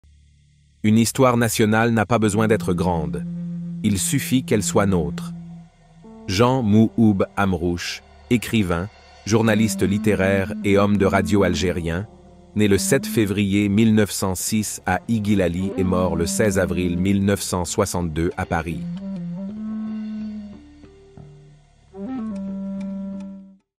«Une histoire nationale n'a pas besoin d'être grande. Il suffit qu'elle soit nôtre» Mouhoub Amrouche
«Une histoire nationale n'a pas besoin d'être grande. Il suffit qu'elle soit nôtre».

Jean-Mouhoub Amrouche, écrivain, journaliste littéraire et homme de radio algérien, né le 7 février 1906 à Ighil Ali et mort le 16 avril 1962 à Paris.